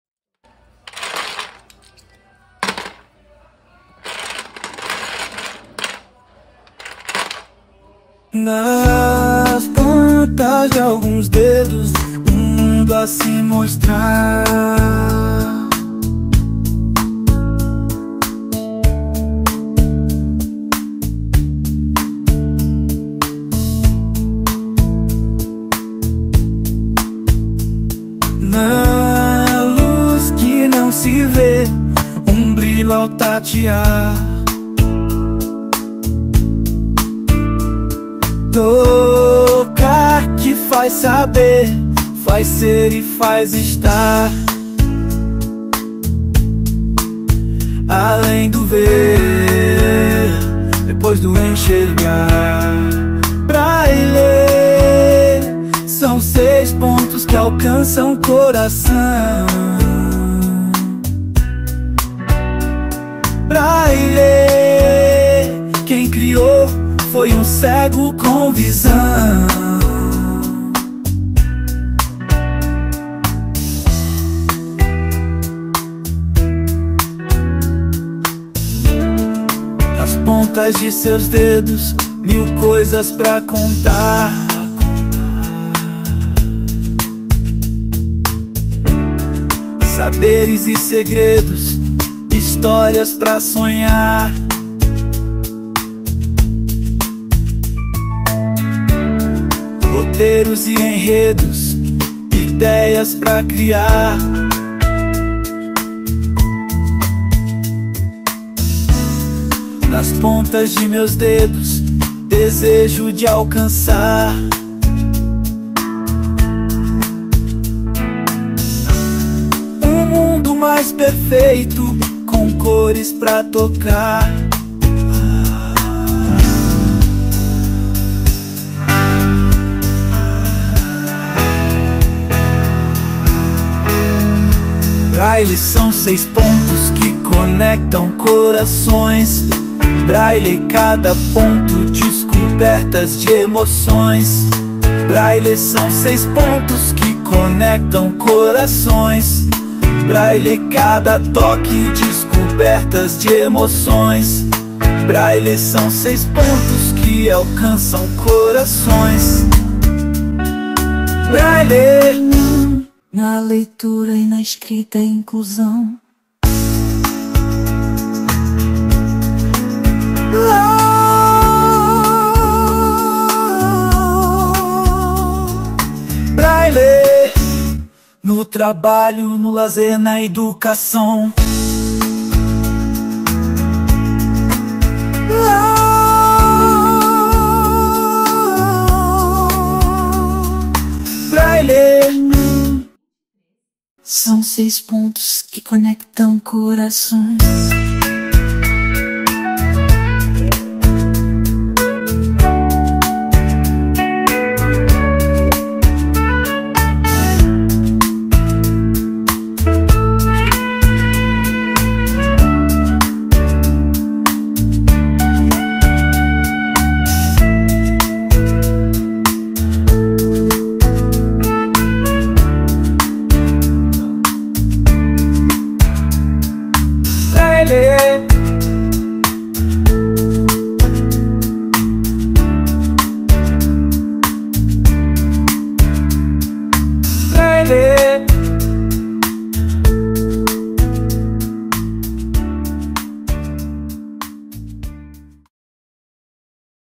Música que fiz praticamente toda com inteligência artificial - AI - Ia
Compus a letra, a inteligência artificial fez o ritimo, a melodia e colocou instrumentos e voz, e eu, fiz a edição final.